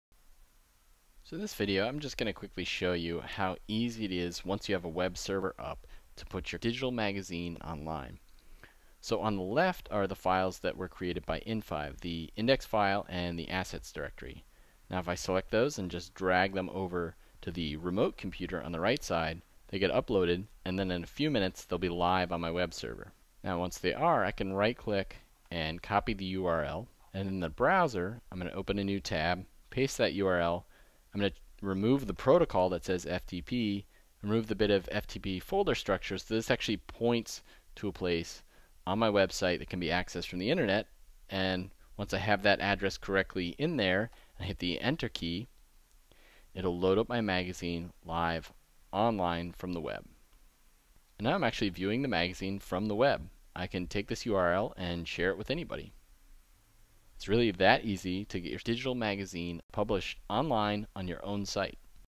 How to Publish a Digital Magazine from InDesign: 8 - Publishing to the Web
Publishing to the Web video from series on Creating a Digital Magazine from InDesign.
Sharing your digital magazine with the world is an easy as copying your in5 files exported from InDesign to your web server.